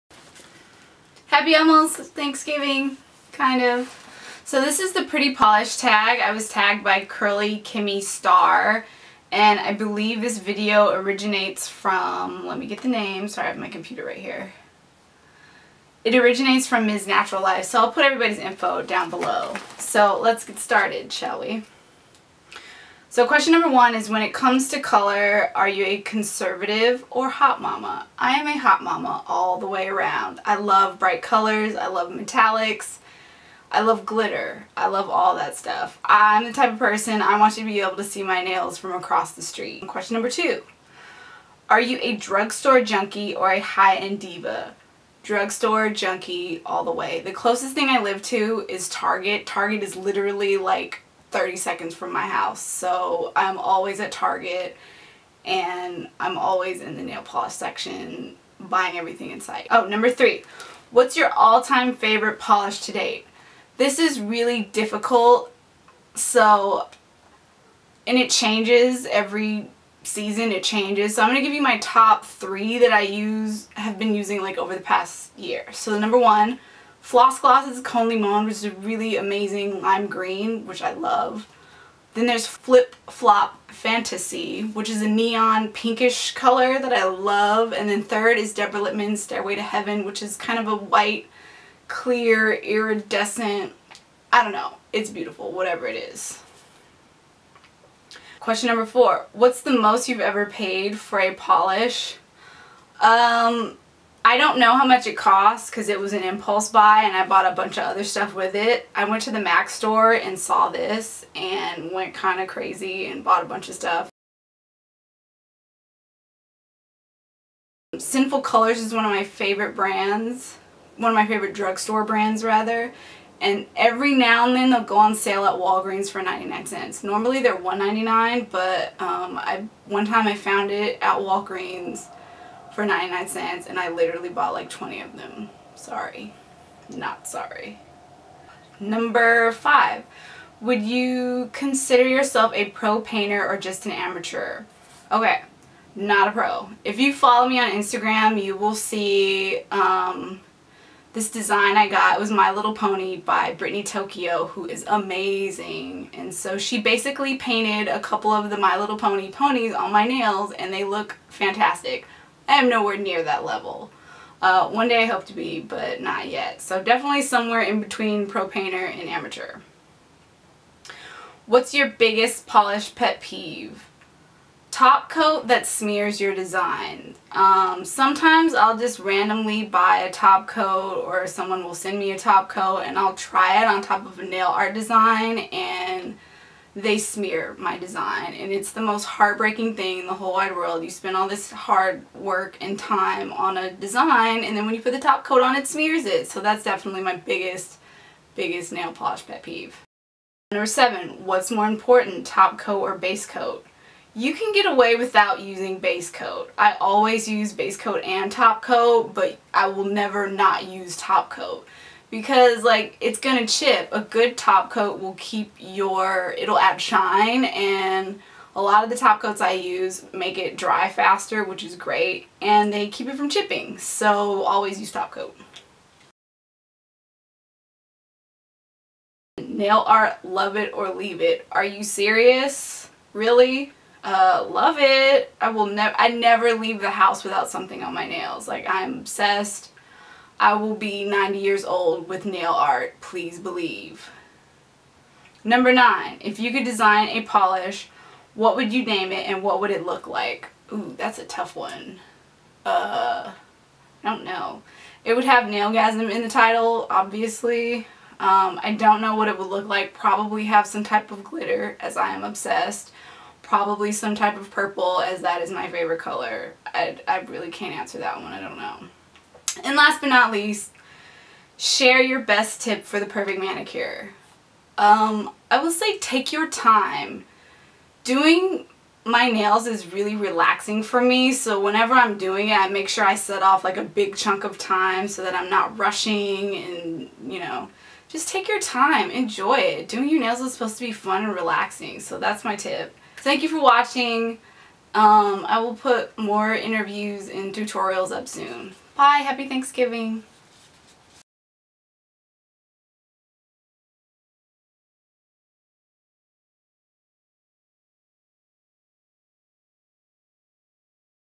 PRETTY POLISH TAG by MzNaturalLife
- I was tagged by CurlyKimmyStar for the "Pretty Polish Tag" by MzNaturalLife. This was so much fun. I LOOOOOOOOOVE talking about nail polish!

1. When it comes to colors, are you conservative or hot mama?
2. Are you a drug store junkie or high end diva?
3. What's your All time favorite polish to date?
4. What's the most you've ever paid for a polish? Least?
5. Would you consider yourself a pro painter or just an amateur?
6. What's your biggest polish pet peeve?
7. What's more important...top coat or base coat?
8. Nail art- love it or leave it?
9. If you could design a polish, what would you name it and what would it look like?
10. Share your best tip for the perfect manicure.

Purchase your DVD copy of NAILgasm: The Nail Art Documentary and more today